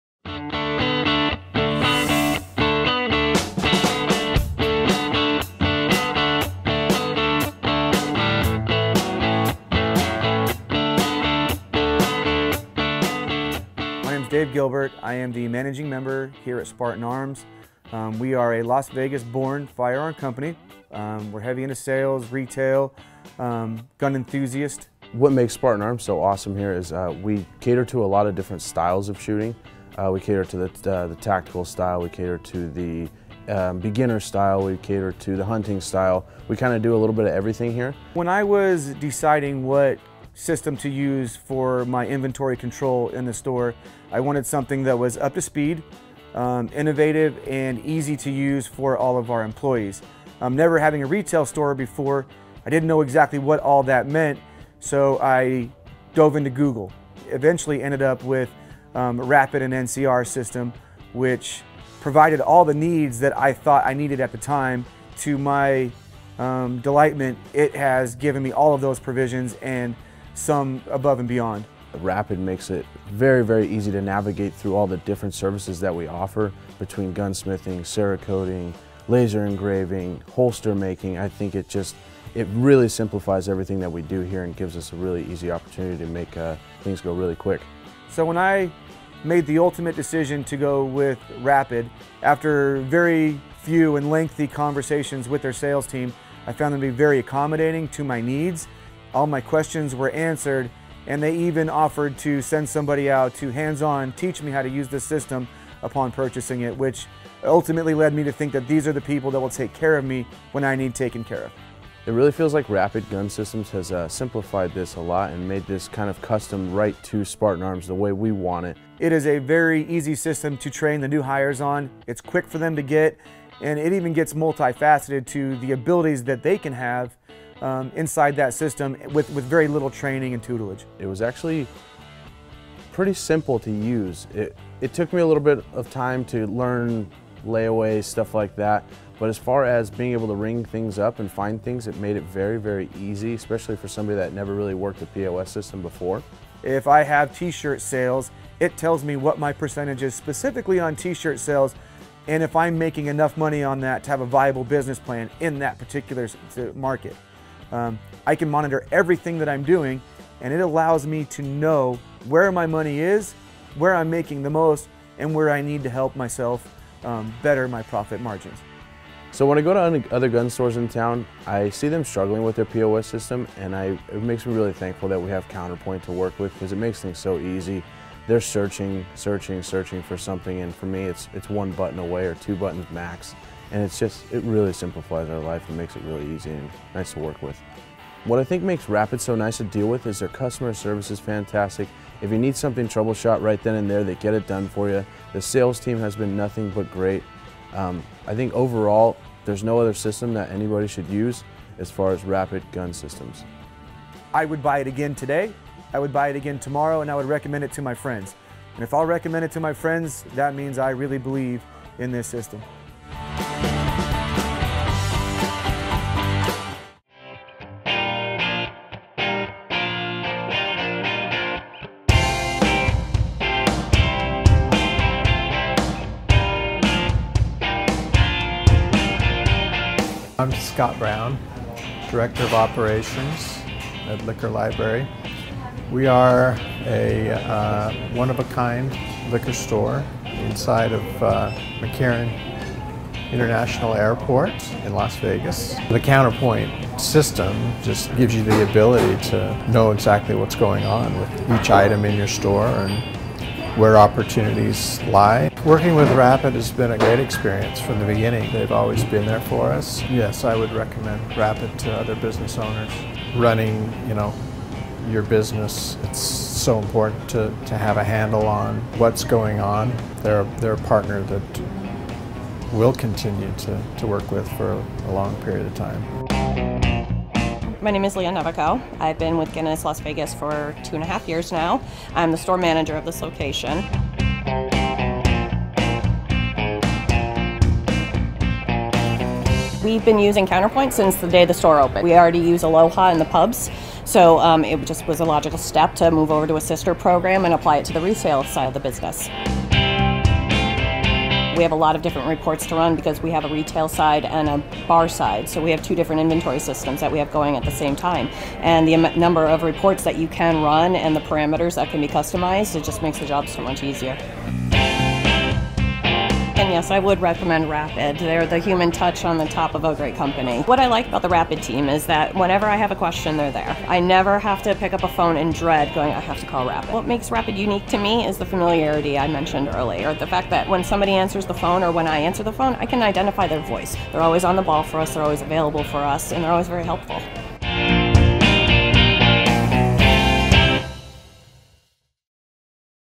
Rapid POS Customer Testimonials, featuring Spartan Arms, Liquor Library, and The Guinness Store
Join Spartan Arms, Liquor Library, and The Guinness Store as they share their experiences working with the team at Rapid POS.